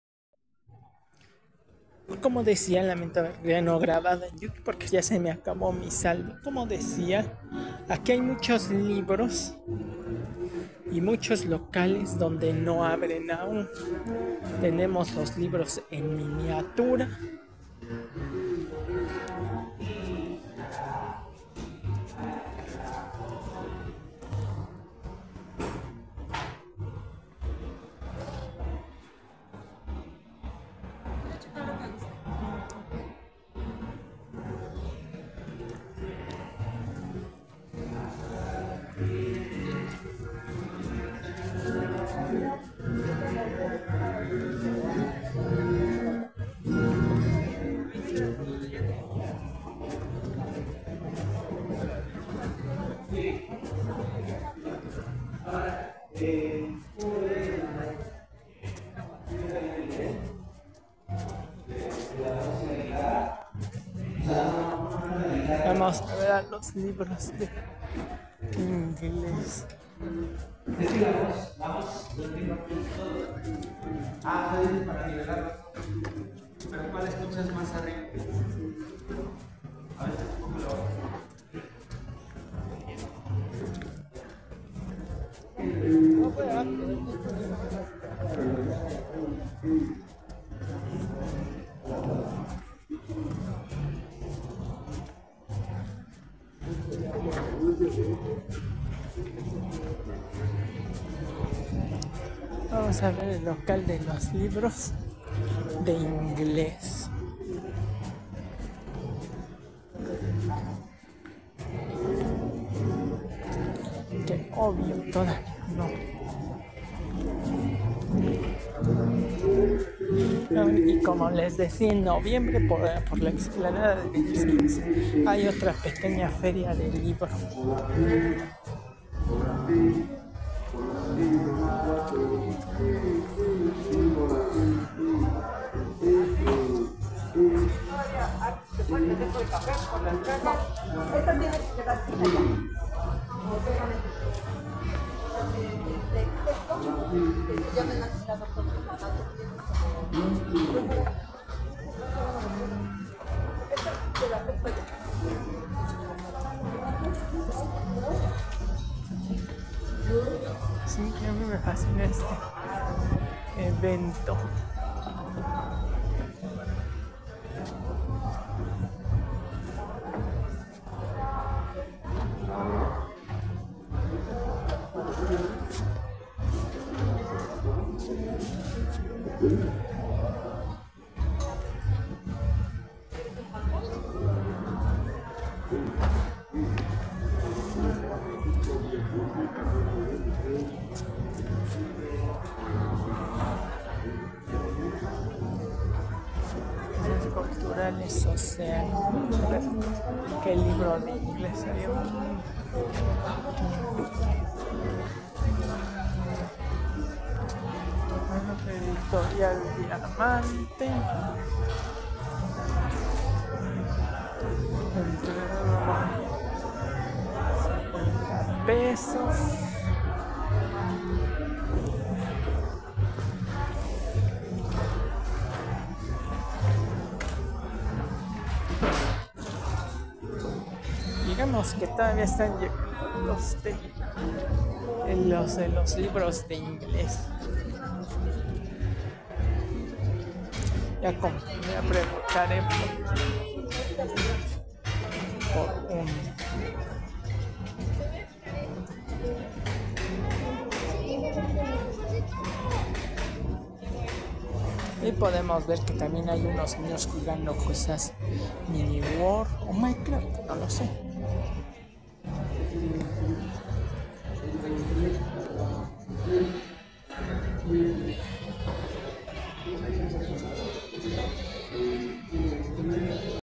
Continuación del recorrido por la feria del libro.
Está es la continuación de este magno evento anual que este año se realiza del viernes 11 de octubre de 2024 al Domingo 20 de octubre de 2024, pero el Sábado 19 de octubre de 2024 es el magno desfile de los "Monumentales Alebrijes": con lo cual da inicio la celebración del "Día de los Muertos" una gran tradición a nivel internacional, que se ha celebrado desde el México Prehispánico y para el Domingo 27 de Octubre de 2024 es el desfile anual del "Día de Muertos"; yo espero que ya todos hallan comprado lo de su ofrenda.